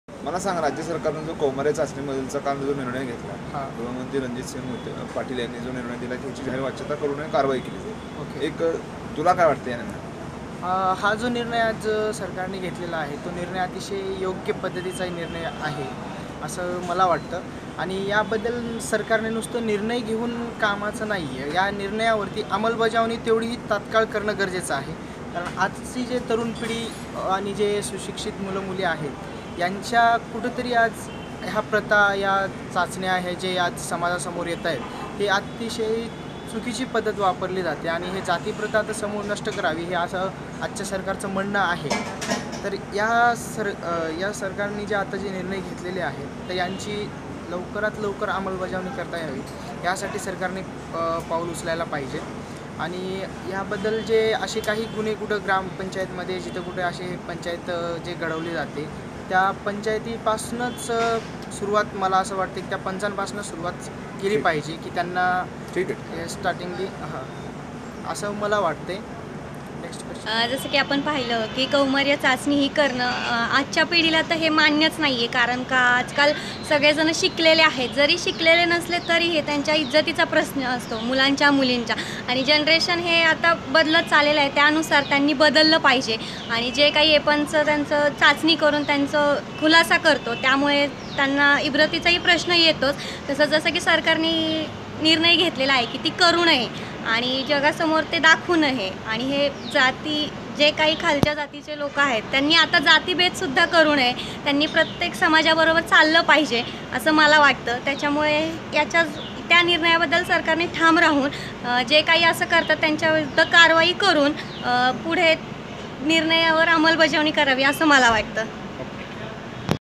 'कौमार्य चाचणीच्या प्रथेविरुद्ध काम करणाऱ्या तरुणांना संरक्षण मिळणार'
पुणे - कंजारभाट समाजातील अनिष्ट प्रथेविरोधात समुपदेशन करुन सुधारणा करणाऱ्या या समाजातील सुशिक्षित तरुणांना आवश्यक ते संरक्षण देण्यात येईल, असे गृह राज्यमंत्री (शहरे) रणजित पाटील यांनी विधान परिषदेत लक्षवेधी सूचनेला उत्तर देताना सांगितले.